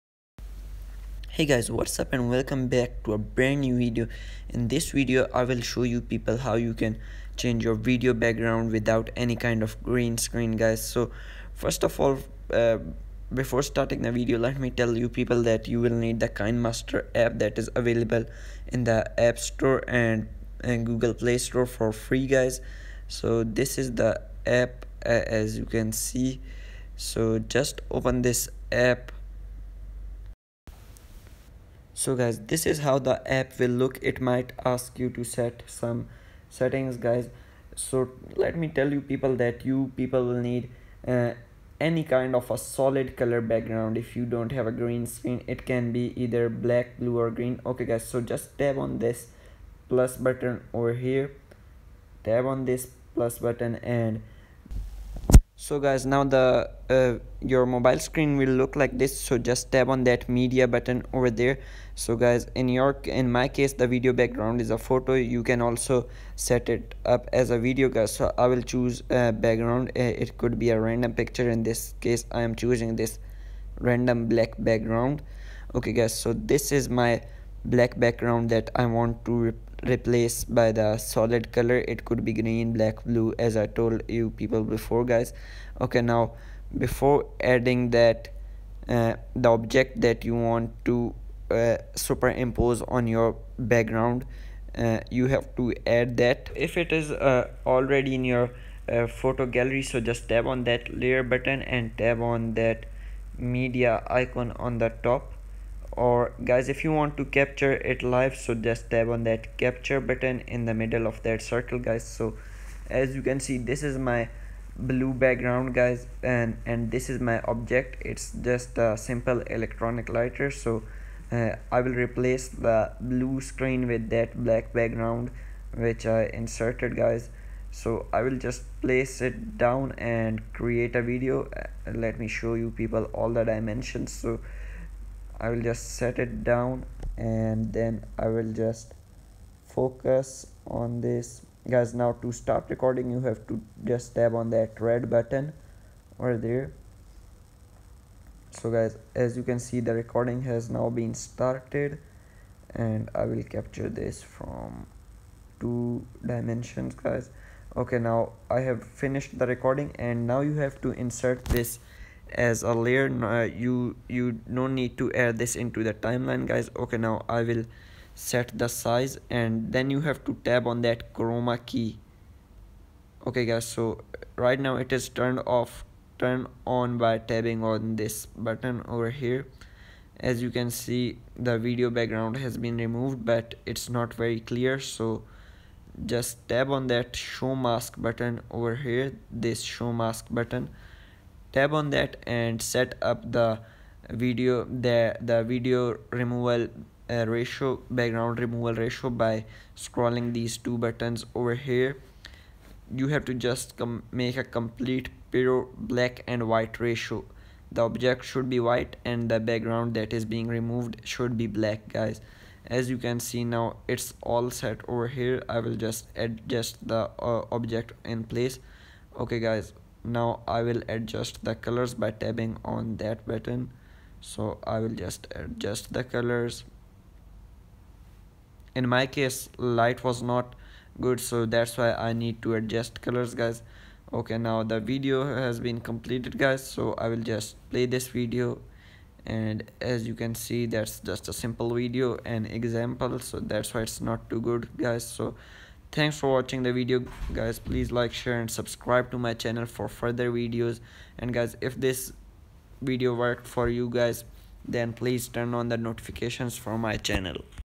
HOW TO CHANGE VIDEO BACKGROUND WITHOUT GREEN SCREEN .
HOW TO CHANGE VIDEO BACKGROUND WITHOUT GREEN SCREEN ON IPHONE ?
How to edit video background.
How to remove video background.
How to change video background without computer.
How to use chroma key effect.
The best app for changing video background.